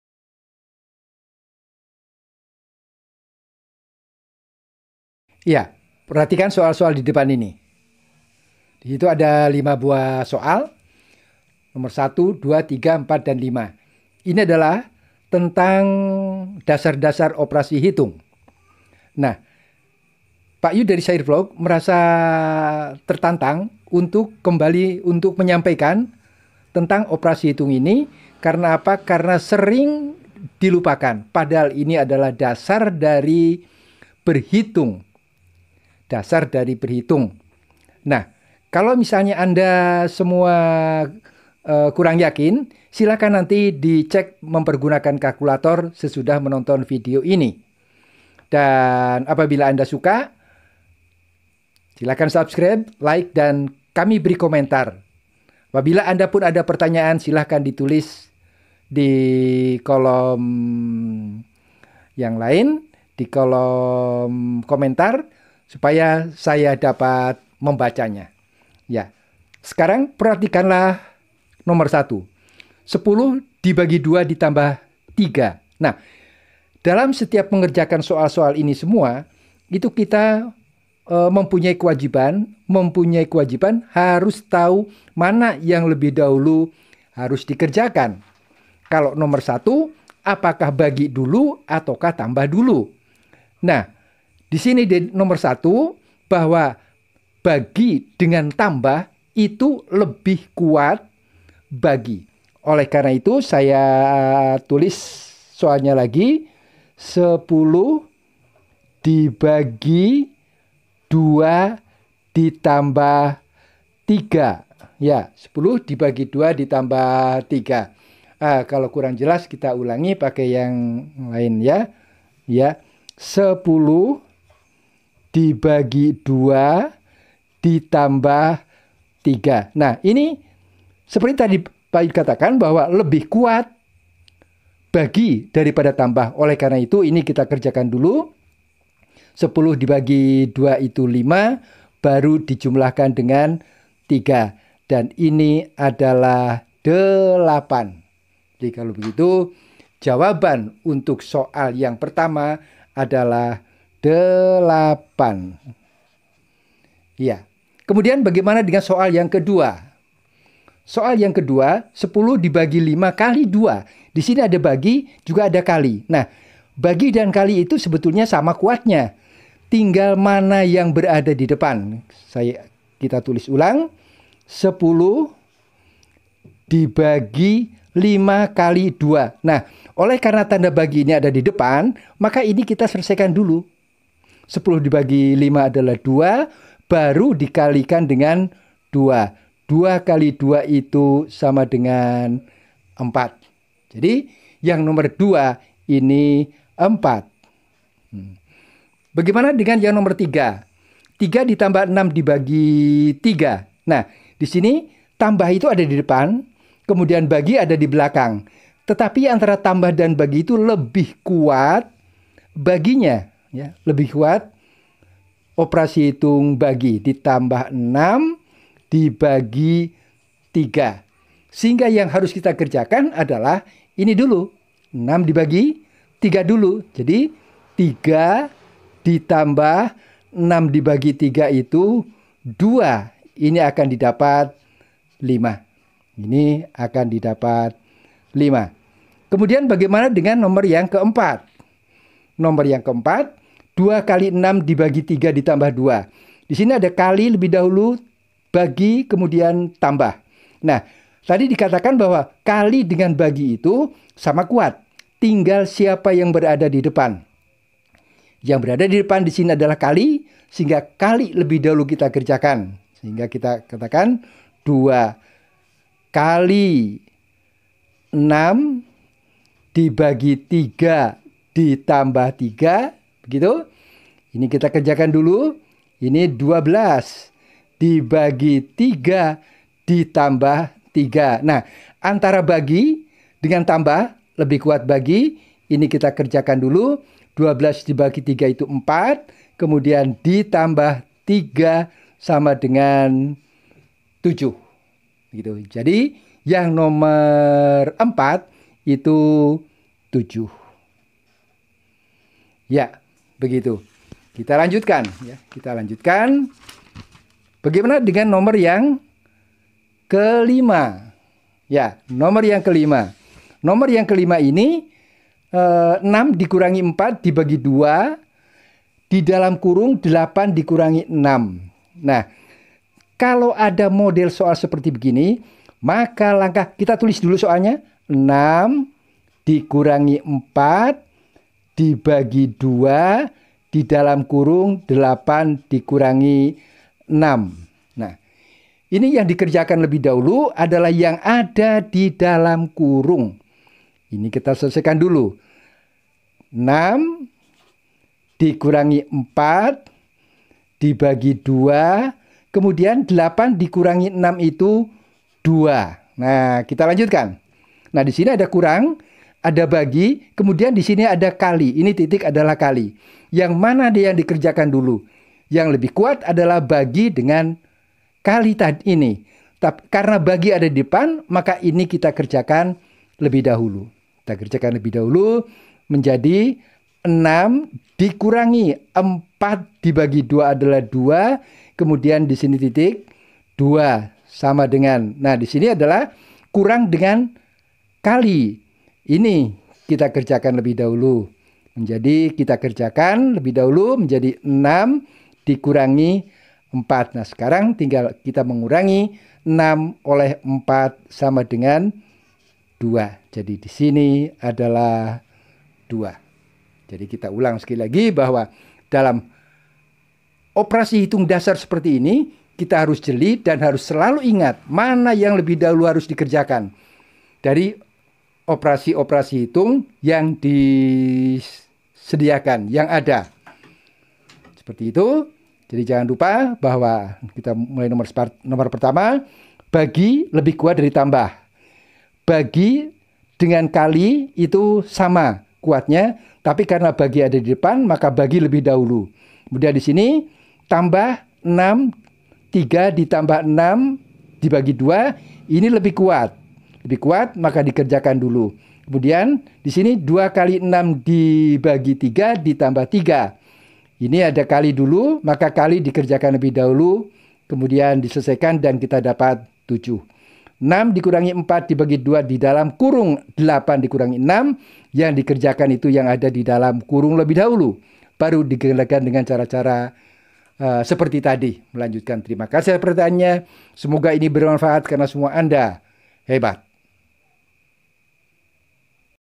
DASAR OPERASI HITUNG || dengan memberikan beberapa operasi hitung pada bilangan
DASAR OPERASI HITUNG, hal yang fundamental dalam berhitung adalah DASAR OPERASI HITUNG, dalam video DASAR OPERASI HITUNG ini diberikan contoh cara menyelesaikan operasi hitung